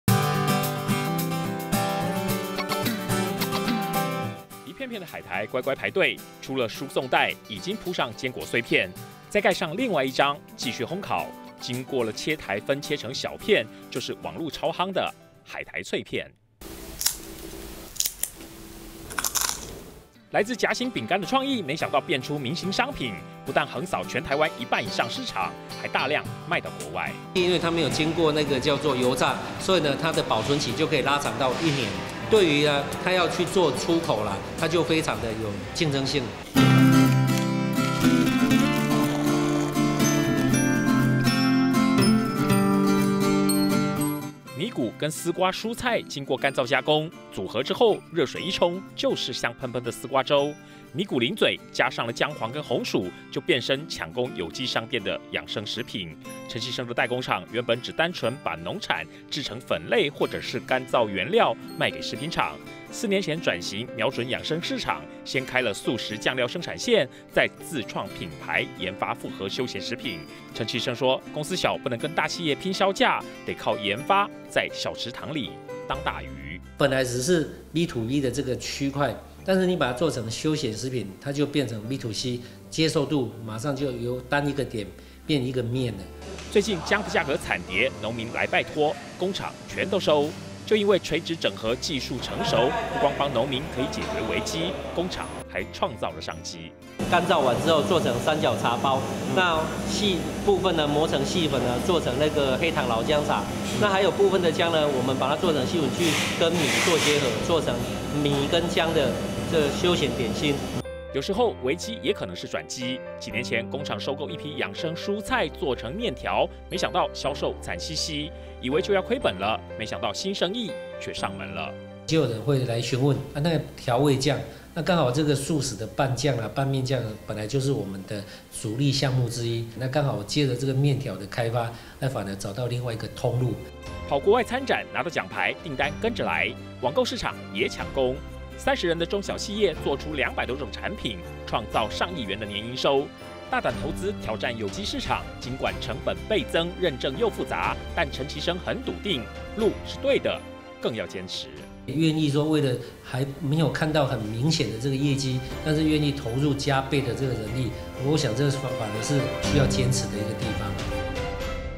20170307中天新聞 雲林囝仔助農民 創品牌轉型成功
雲林是傳統農產之鄉，農民最頭疼的，就是產量過剩，血本無歸！雲林子弟陳麒升，在家鄉設代工廠，收購農產，加工成食品原料，但代工一做26年，漸漸發現，大量生產，拚不過財團，得「在小池塘裡當大魚」，四年前自創品牌，運用純熟的農產加工技術，再創意研發獨家的休閒零嘴跟沖泡食品，最近網購超夯的堅果海苔脆片，絲瓜沖泡粥等，成功攻佔有機商店，還獨佔全台過半市場，旗下產品多達200多種，每年賺進一億元營收，也幫了家鄉的農民！